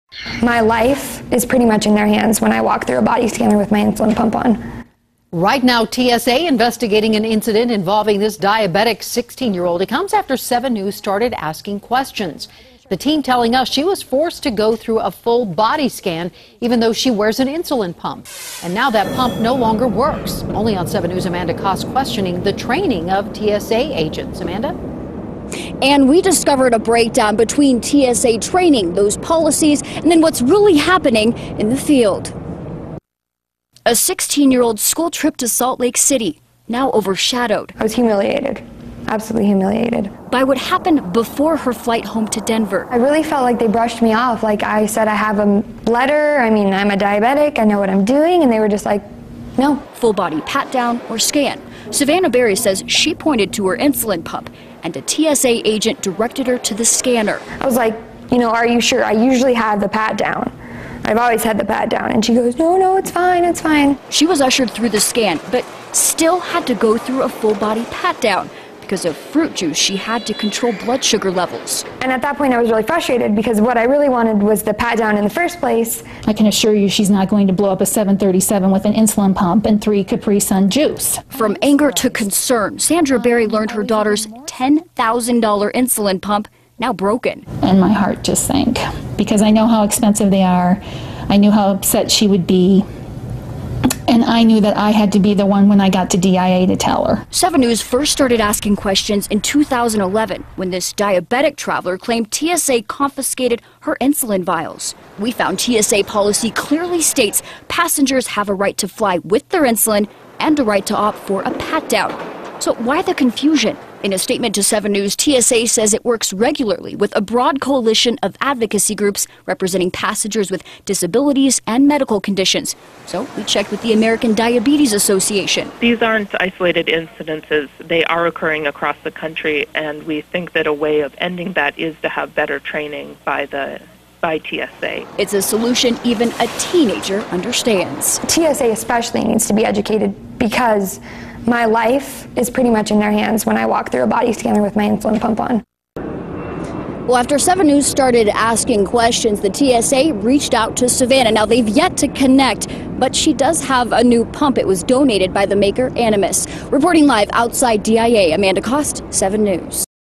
Teen Blames TSA for Broken Insulin Pump
Diabetic teen says TSA instructions ruined her $10,000 insulin pump.  7NEWS reporter Amanda Kost discovered a breakdown between TSA training and what really happens in the field.